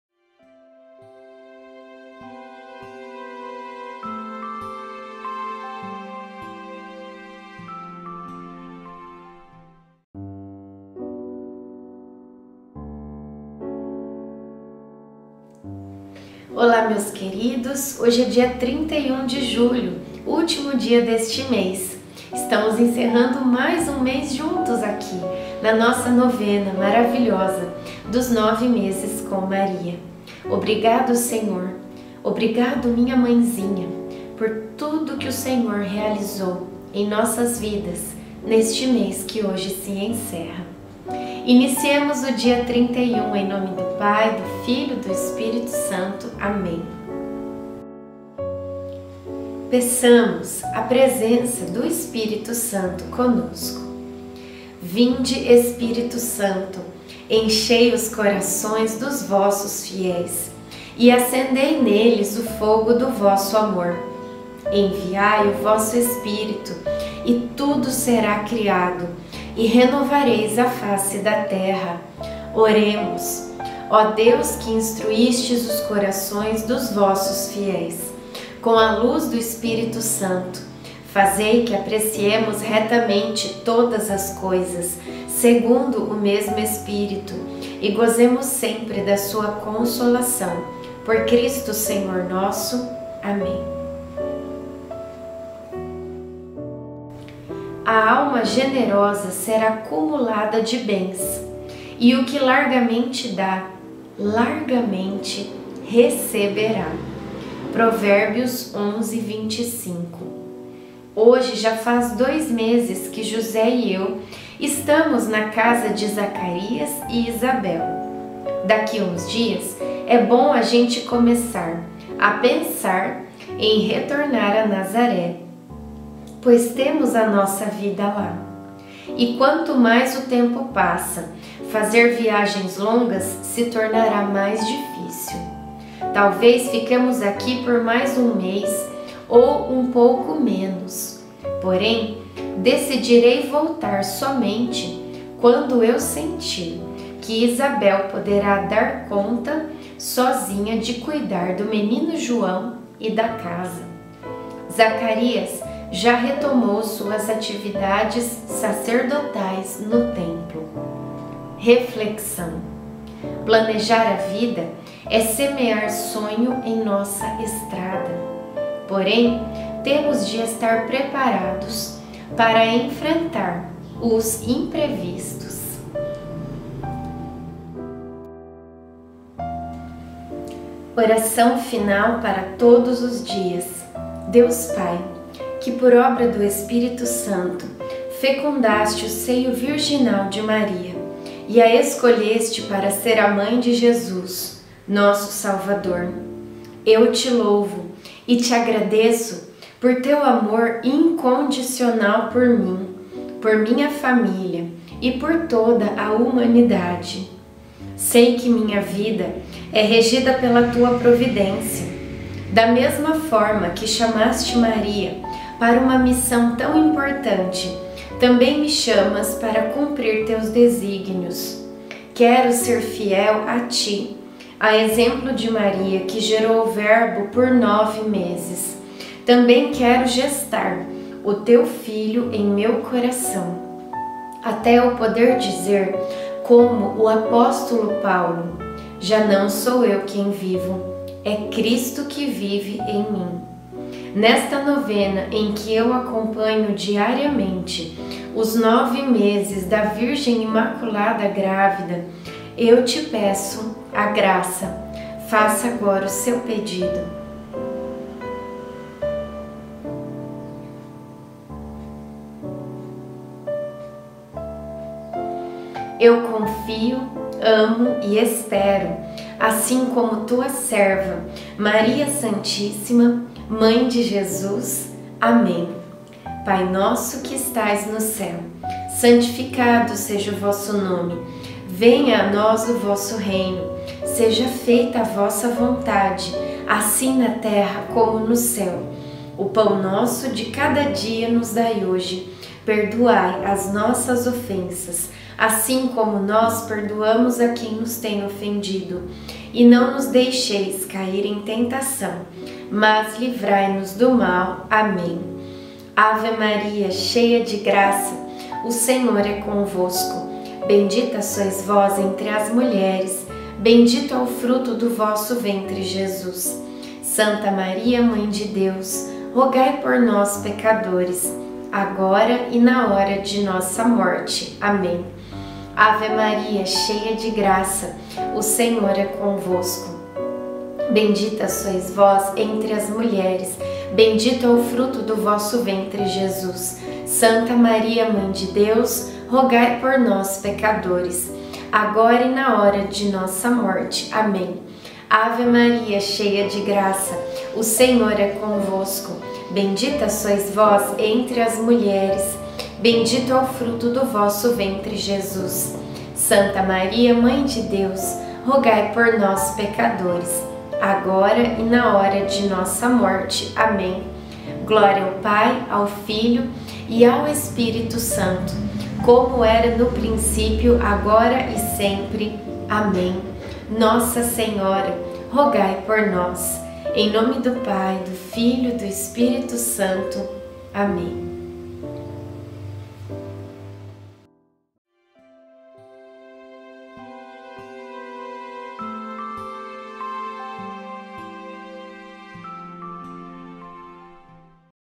NOVENA 9 MESES COM MARIA - 31/07
31 de julho

A alma generosa será cumulada de bens; e o que largamente dá, largamente

receberá. (Provérbios 11,25)

Hoje já faz dois meses que José e eu estamos na casa de Zacarias e Isabel. Daqui
uns dias, é bom a gente começar a pensar em retornar a Nazaré, pois temos nossa
vida lá. E, quanto mais o tempo passa, fazer viagens longas se tornará mais difícil.
Talvez fiquemos aqui por mais um mês ou um pouco menos. Porém, decidirei
voltar somente quando eu sentir que Isabel poderá dar conta sozinha de cuidar do
menino João e da casa. Zacarias já retomou suas atividades sacerdotais no Templo.

Planejar a vida é semear sonho em nossa estrada.
Porém, temos de estar preparados para enfrentar os imprevistos.

doações espontâneas
Deus abençoe!
Link: (388) Patrícia Acosta Cozinha Comigo - YouTube